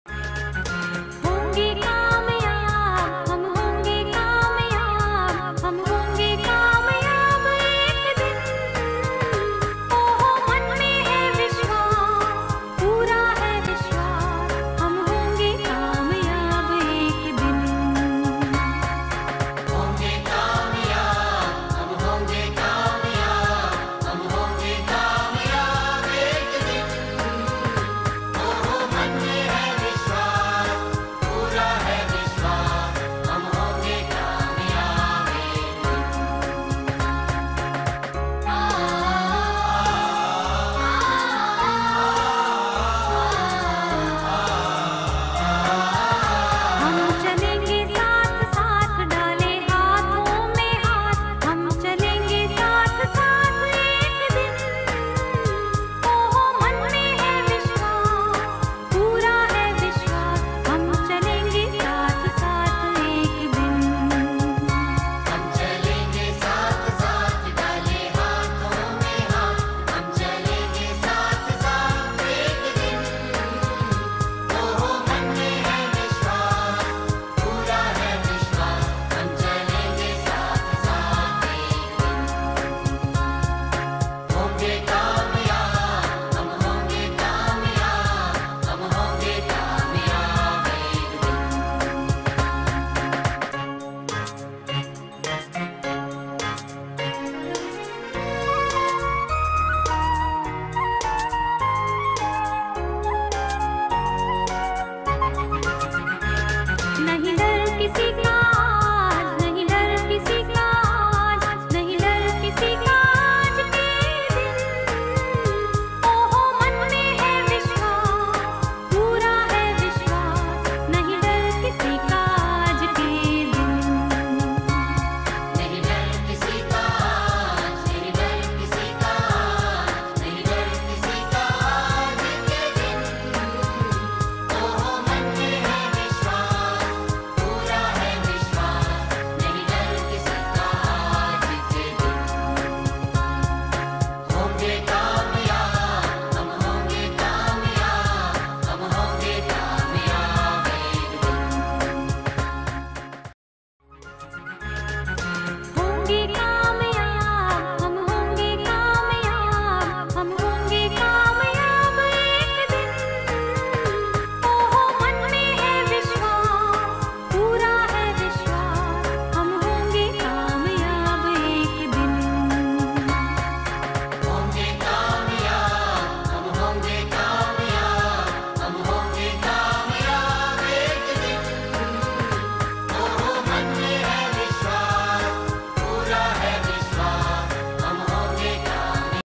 GOVERNMENT GUJARATI HIGH SCHOOL- BANTWA IDEAL SCHOOL INTRODUCTION
ગુજરાત રાજ્યની સરકારી શાળાઓ પૈકી એક આદર્શ હાઇસ્કુલ એટલે જુનાગઢ જીલ્લામાં બાંટવા ખાતે આવેલી સરકારી ગુજરાતી હાઇસ્કુલ..
જુઓ શાળાની સર્વાંગી પ્રવૃત્તિઓ અને અદ્યતન સુવિધા..